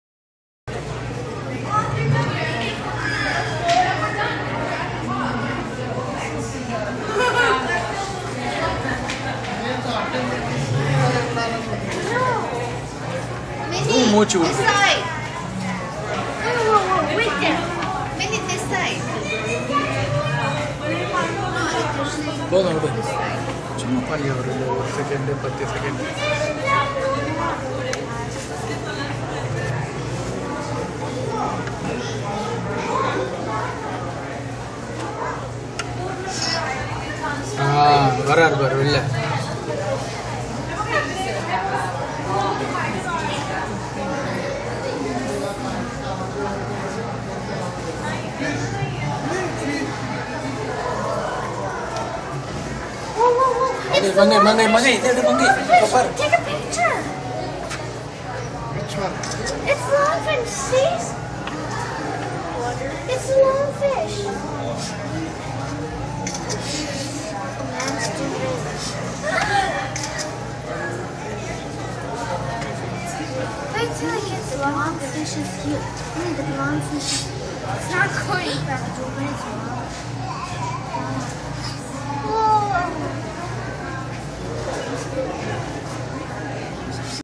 Green long fish
When I went to an Aquarium, saw this fish for the first time and thought of recording it for others to see